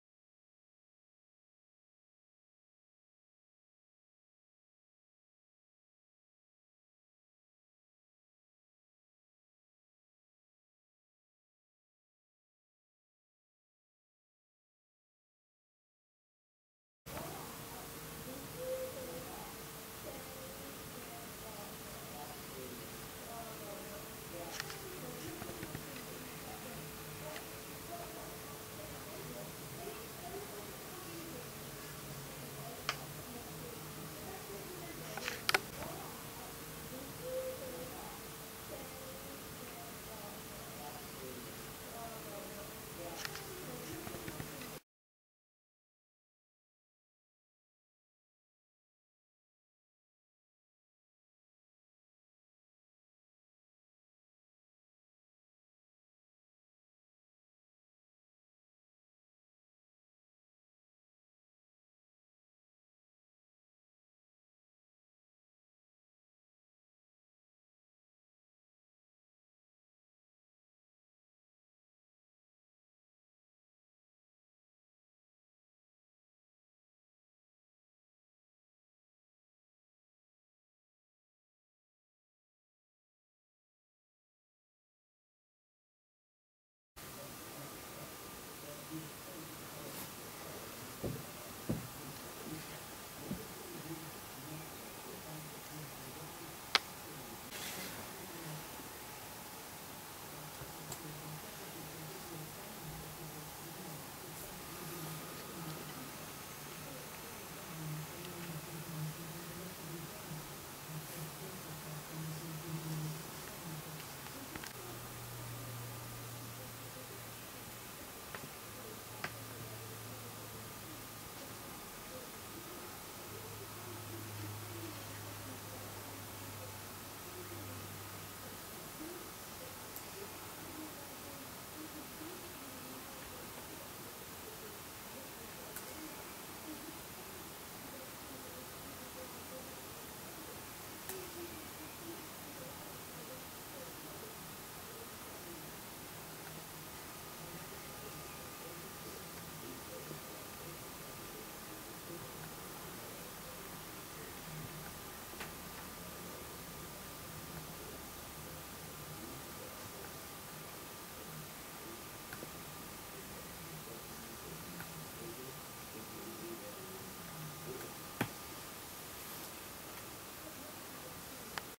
Cyberlink Power Director 9, Black Box/Crashing Issues SOLVED!!!
After buying via download POWER DIRECTOR 9, I kept having 'Crashing' issues at certain times.This was really doing my head in as i couldnt finish my videos propperly. However via cyberlinks website i found the solution so i thought i'd share it with other Power Director users.